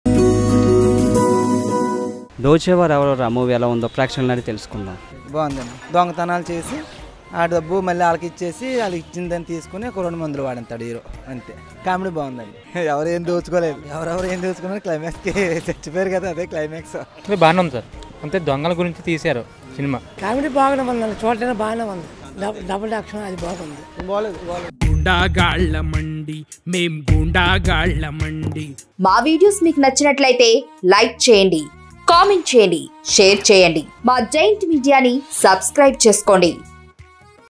Dochevaarevarura Rajahmundry Public Talk || Siva Nageswar Rao, Ajay Ghosh, Bithiri Sathi, Malavika
Dochevaarevarura Rajahmundry Public Talk  || Siva Nageswar Rao, Ajay Ghosh, Bithiri Sathi, Malavika, Pranava Chandra @giantmediachannel